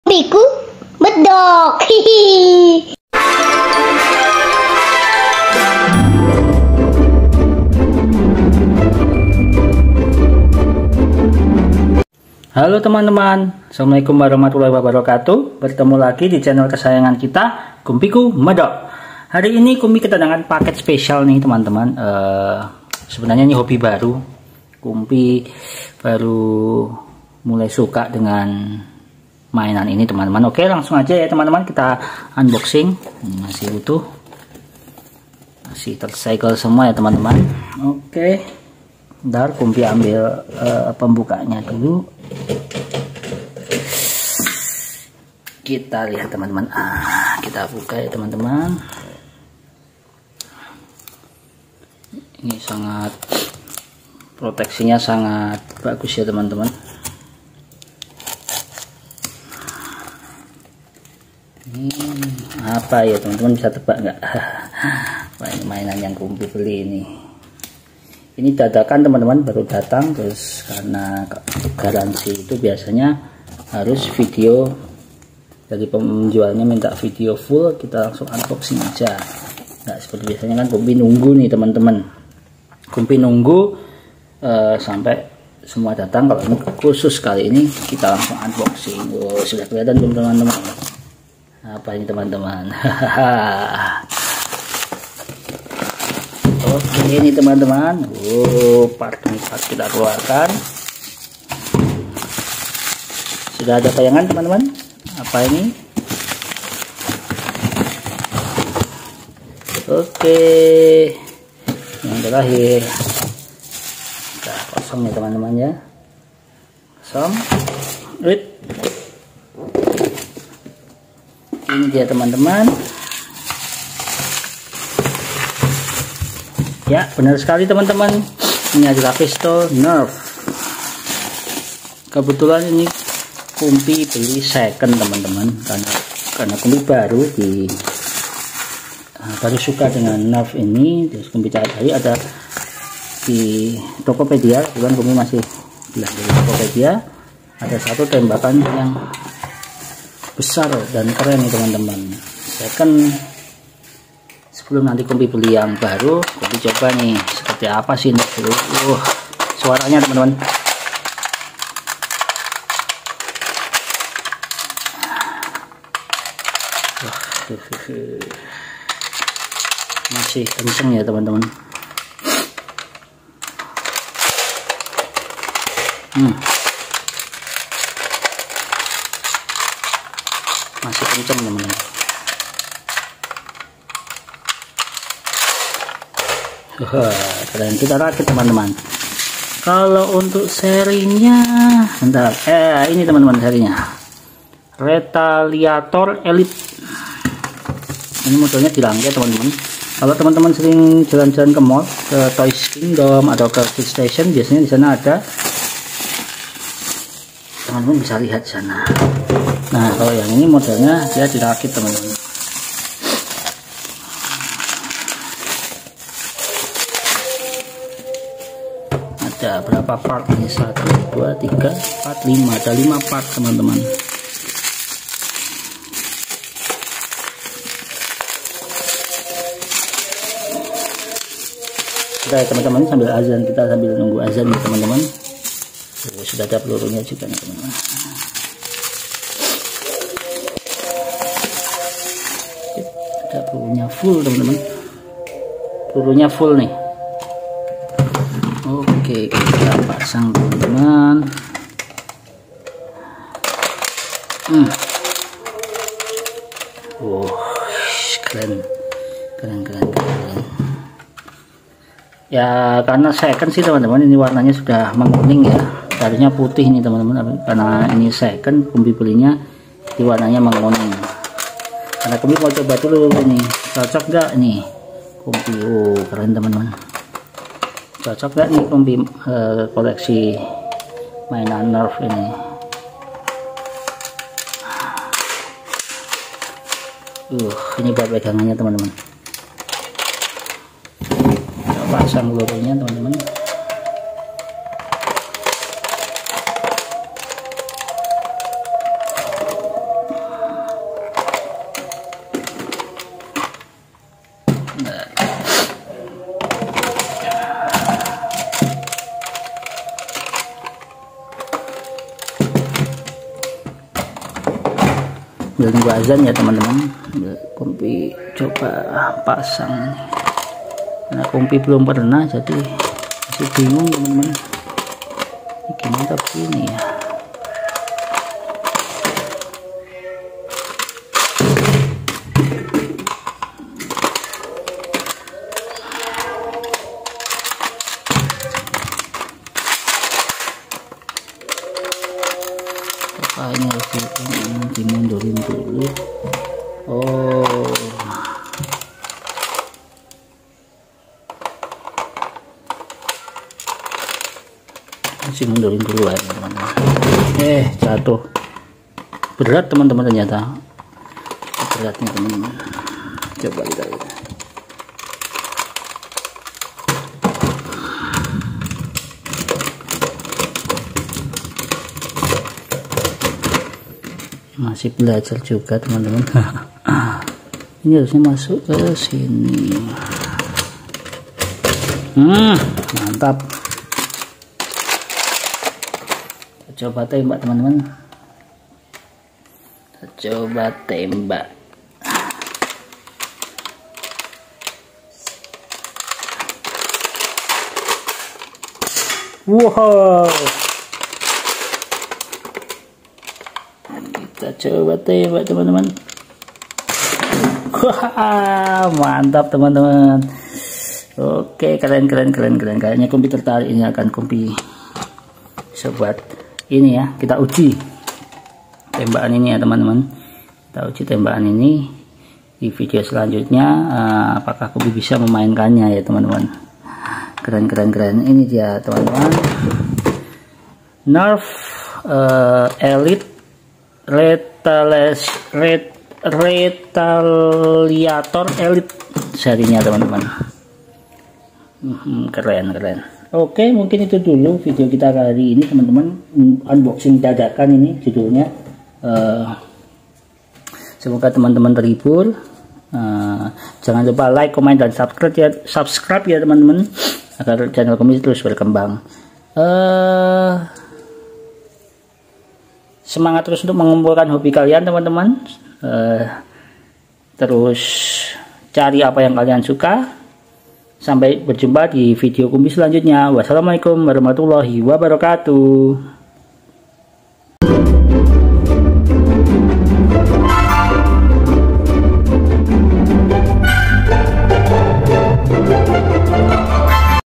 Nerf retaliatior elite 2012 | Koleksi senjata pertama kumpi | REVIEW MAINAN RANDOM
Assalamualaikum Teman-Teman!! Selamat datang di Youtube Channel Kumpiku medoK! Bismillah! Do'ain Kumpi ya !
Hallo teman teman ^_^
hari ini koleksi mainan yang kita review :
Name : Nerf
Series :  Retaliatior
Brand : Hasbro
Country : -
Rilis : 2012

The Retaliator is a slide-action clip system blaster advertised as a "4-in-1" blaster. Using the included accessories, the Retaliator can be customized to the user's desire, allowing them to create their own blaster.

Terimakasih teman - teman ! jangan lupa